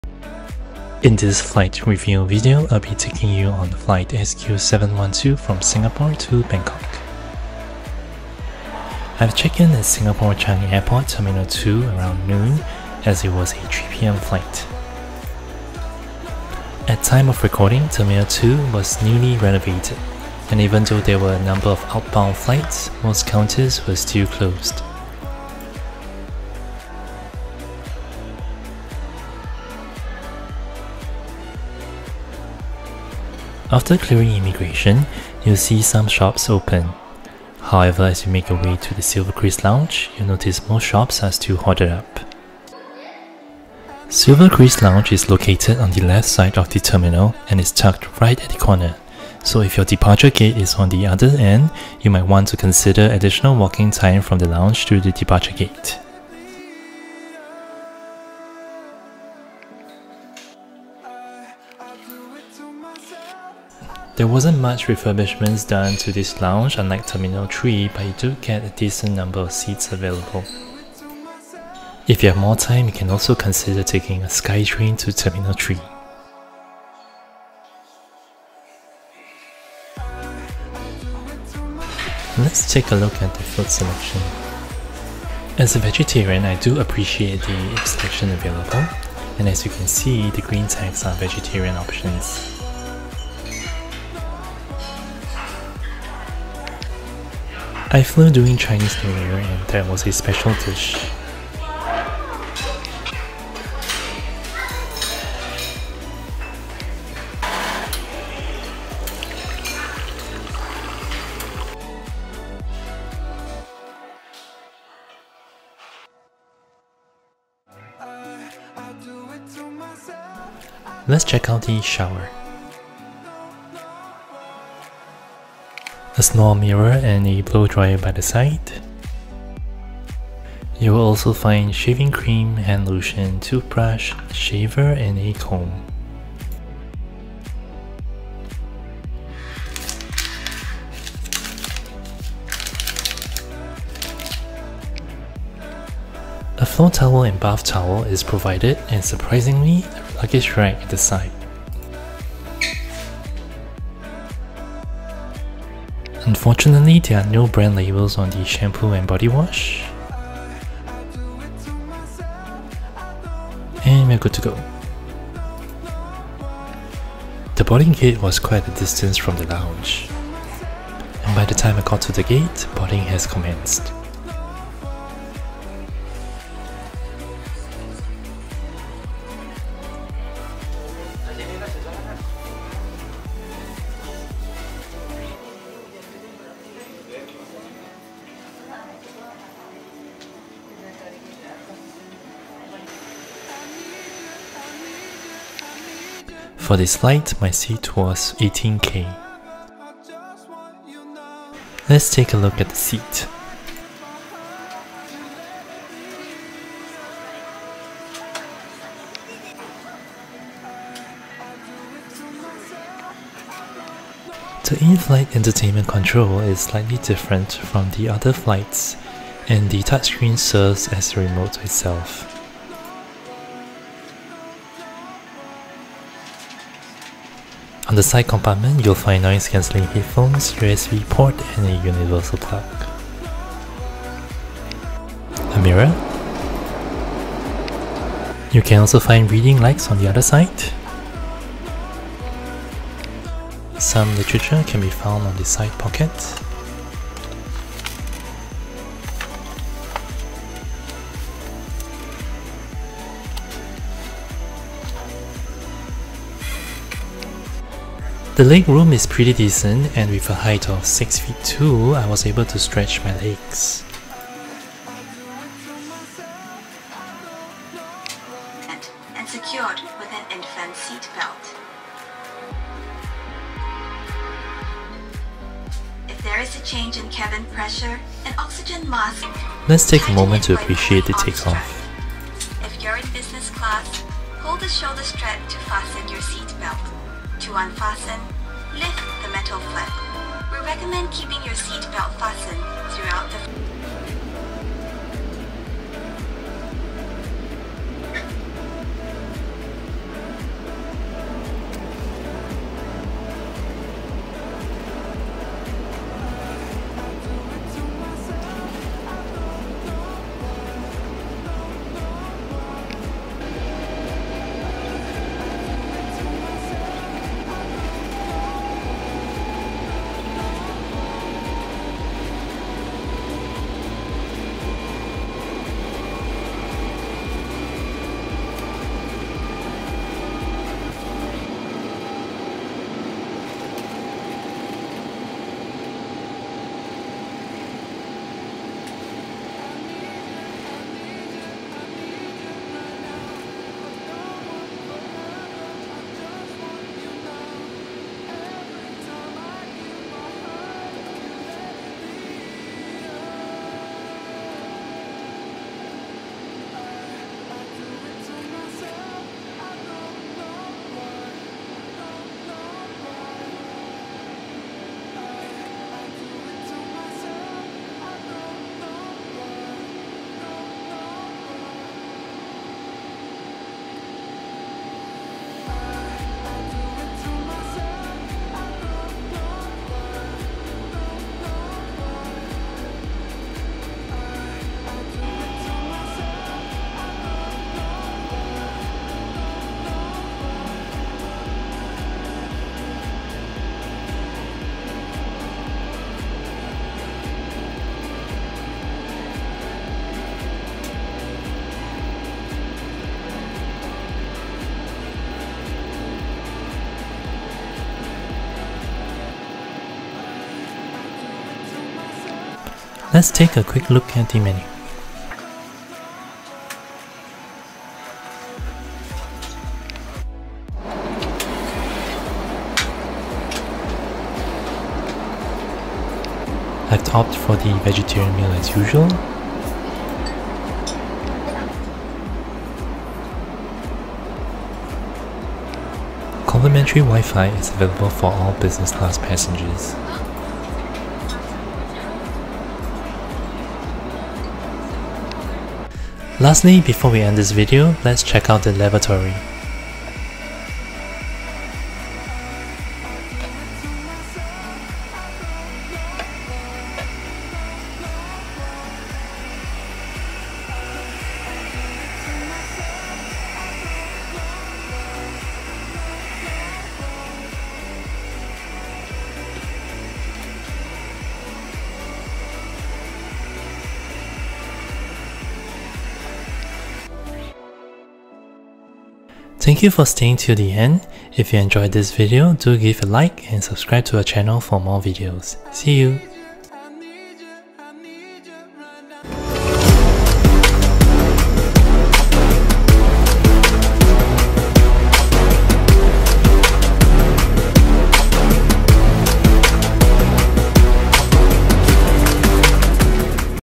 ✈️🇸🇬🇹🇭 SQ712 Singapore ✈︎ Bangkok, Singapore Airlines Business Class SIN-BKK A350-900
In this flight review, I flew Singapore Airlines SQ712 (Airbus A350-900) from Singapore to Bangkok (SIN-BKK) 🇸🇬🇹🇭 and we will bring you on the flight experience of Business Class.

This video of the flight SQ712 from SIN-BKK was taken on 22 January 2023. There may be changes and any information presented in the video may change accordingly.

00:00 Departure Hall / Check-in / Immigration
01:01 SilverKris Lounge
03:17 Seat Tour
04:40 Take off
06:54 Menu / Meal
07:26 Lavatory